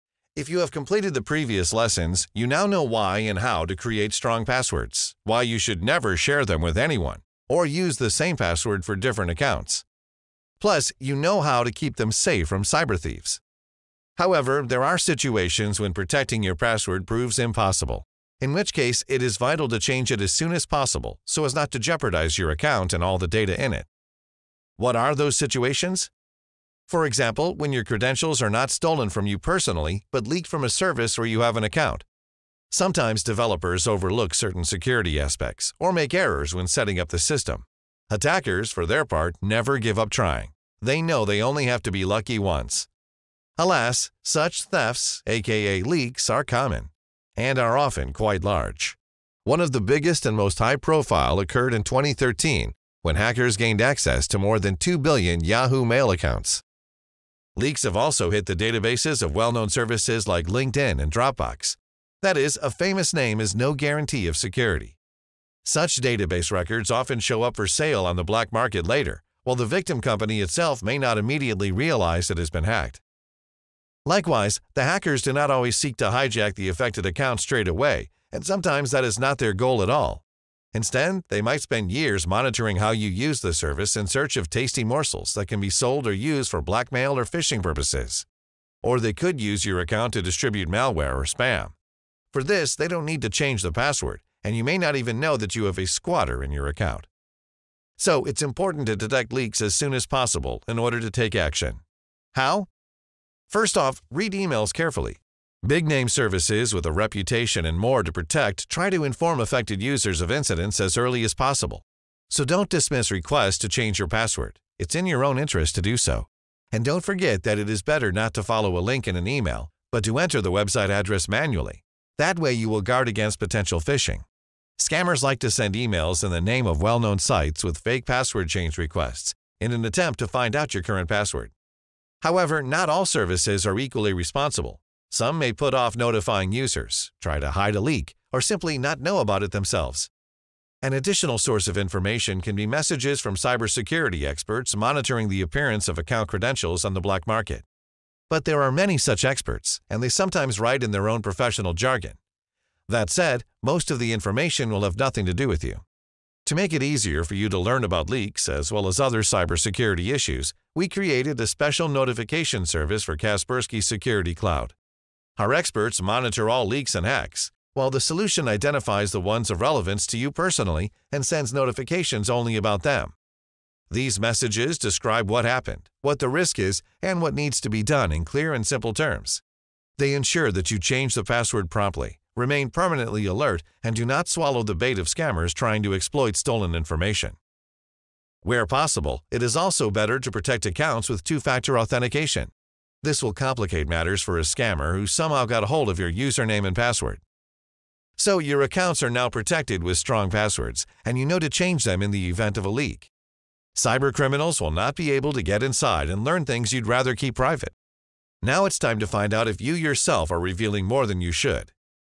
Data leaks and when to change your password
There are situations when protecting your password proves impossible, in which case you must change it as soon as possible so as not to jeopardize your account and all the data in it.
For example, when your credentials are not stolen from you personally, but leaked from a service where you have an account. Alas, such thefts (aka leaks) are common. And are often quite large.